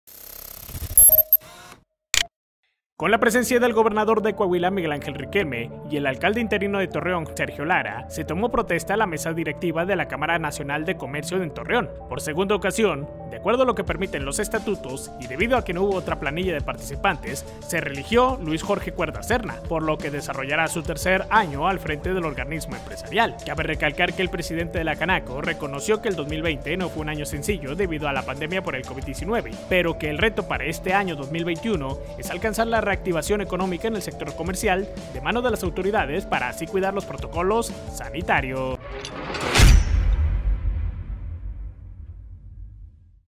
Reeligen a Luis Cuerda al frente de Canaco Torreón
Con la presencia del gobernador de Coahuila, Miguel Ángel Riquelme, y el alcalde interino de Torreón, Sergio Lara, se tomó protesta a la mesa directiva de la Cámara Nacional de Comercio (Canaco) en Torreón.